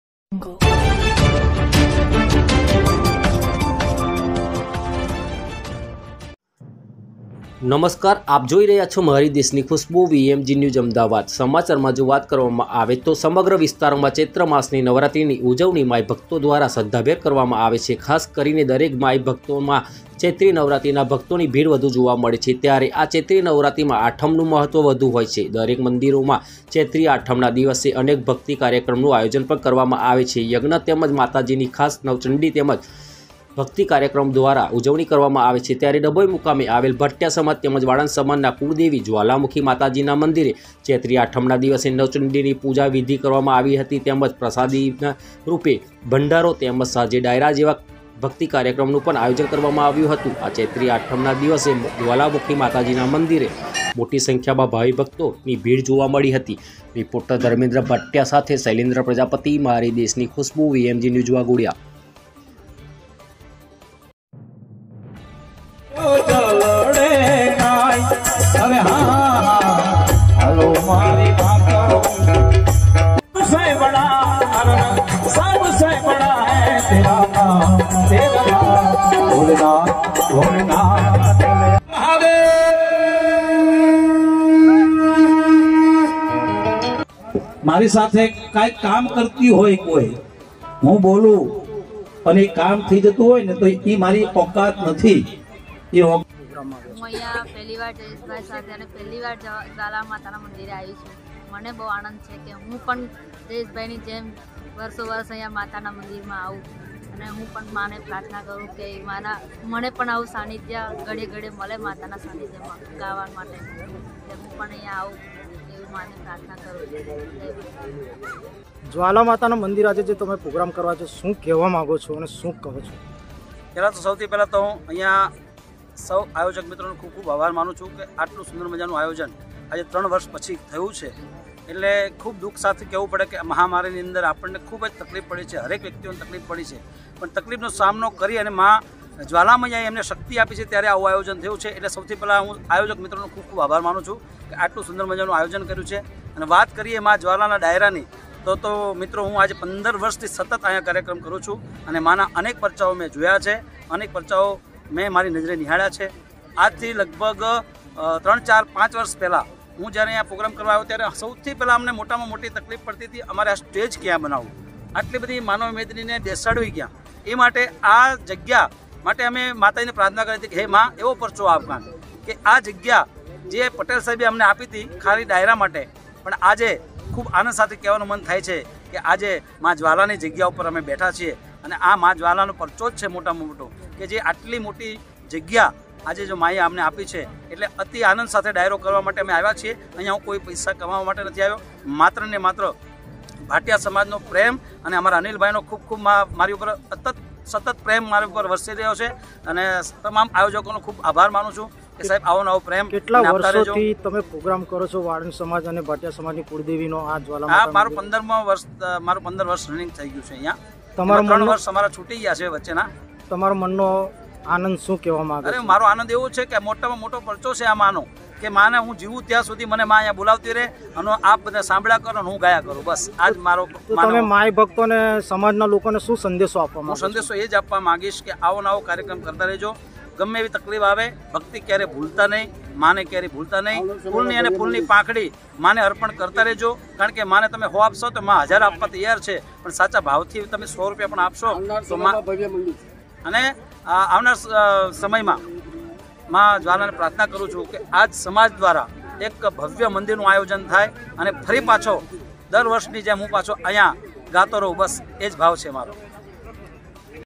વાઘોડિયા ચૈત્રી આઠમના દિવસે જ્વાલામુખી માતાજીના મંદિરે મોટી સંખ્યામાં ભાવિ ભક્તોની ભીડ જોવા મળી હતી.
વાઘોડિયા સહિત
સમગ્ર વિસ્તારોમાં ચૈત્ર માસની નવરાત્રી ની ઉજવણી માઇ ભકતો દ્વારા શ્રદ્ધાભેર કરવામાં આવી

 ખાસ કરીને દરેક માંઇ મંદિર માં ચૈત્રી નવરાત્રિમાં ભક્તોની ભીડ વધુ જોવા મળે છે. ત્યારે આ ચૈત્રી નવરાત્રિમાં આઠમ નું મહત્વ વધુ હોય છે. દરેક મંદિરોમાં ચૈત્રી આઠમના દિવસે અનેક ભક્તિ કાર્યક્રમનું આયોજન કરવામાં આવે છે. યજ્ઞ તેમજ માતાજીની ખાસ નવચંડી તેમજ અનેક ભકિત કાર્યક્રમો દ્વારા ઉજવણી કરવામાં આવે છે.
ત્યારે ડભોઇ મુકામે આવેલ ભાટિયા તેમજ વાળંદ સમાજ ના કુળદેવી જ્વાલામુખી ના મંદિરે ચૈત્રી આઠમના દિવસે  નવચંડી ની પૂજા વિધિ કરવામાં આવી હતી. તેમજ પ્રસાદી રૂપે ભંડારો તેમજ સાંજે ડાયરા જેવા ભક્તિ કાર્યક્રમોનું પણ આયોજન કરવામાં આવ્યું હતું. આ ચૈત્રી આઠમ ના દિવસે જ્વાલામુખી માતાજીના મંદિરે મોટી સંખ્યામાં ભાવિક ભક્તોની ભીડ જોવા મળી હતી.

રીપોર્ટર. ધમેન્દ્ર ભાટિયા સાથે શૈલેન્દ્ર પ્રજાપતિ વાઘોડિયા